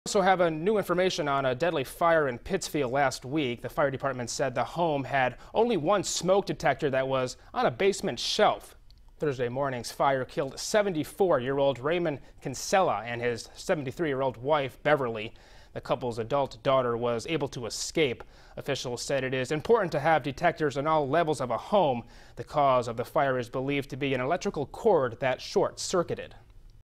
Cause of deadly Pittsfield fire identified
The Fire Department said a Pittsfield home in which an elderly couple died in a fire had only one smoke detector, which was on a basement shelf.